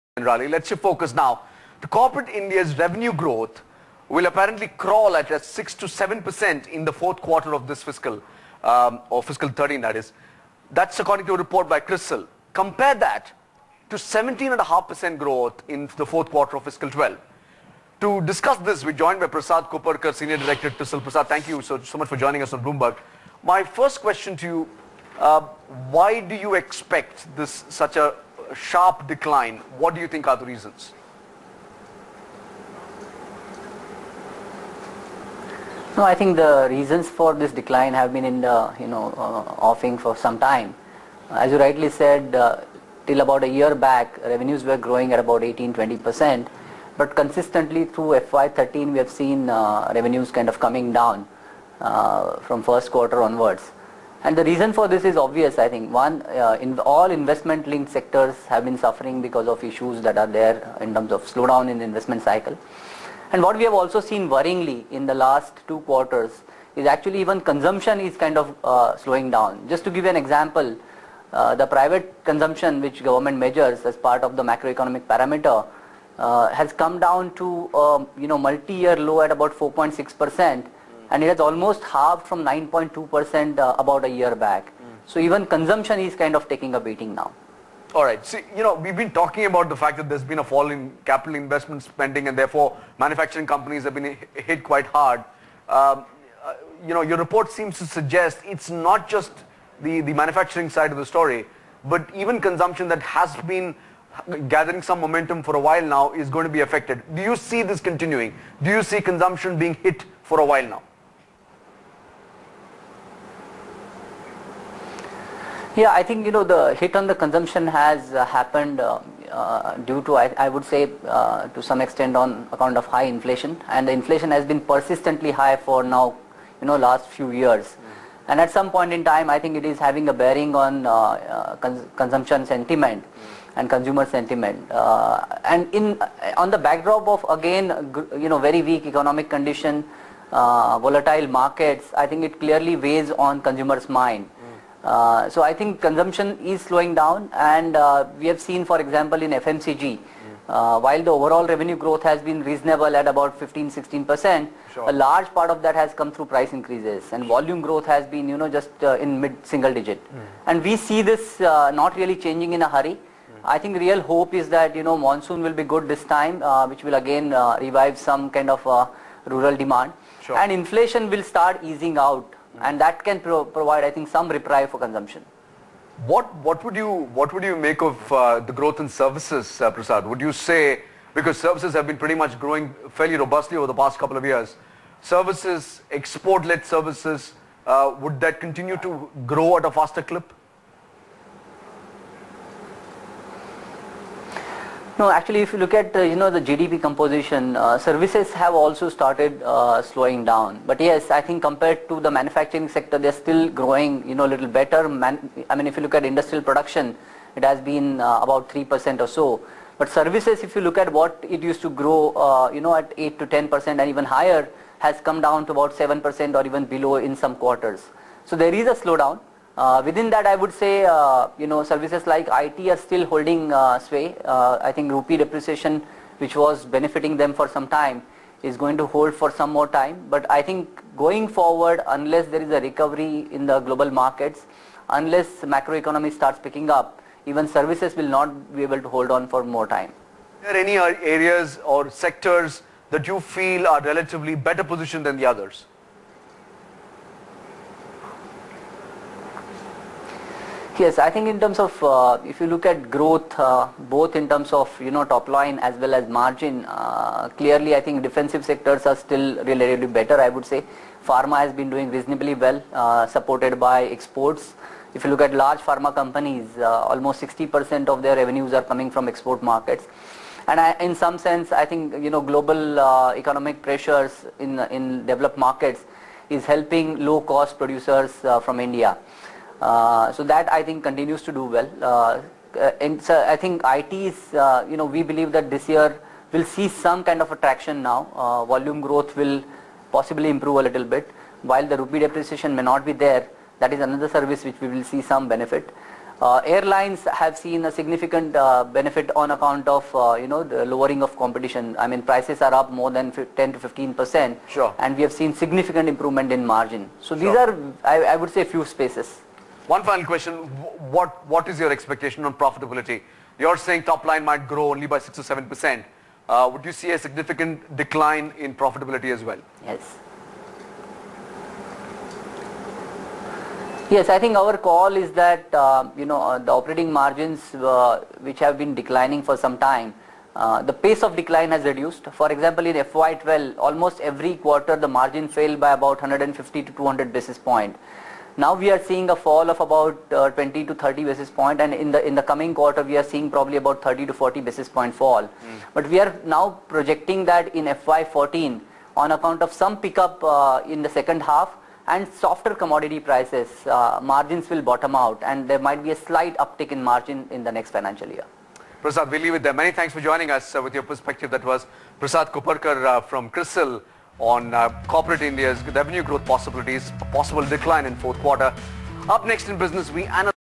A discussion on CRISIL Research's press release on Corporate Profitability on Bloomberg TV India
Prasad Koparkar, Senior Director, Industry and Customised Research, CRISIL Research on Bloomberg TV India in discussion on CRISIL Research's press release on Corporate Profitability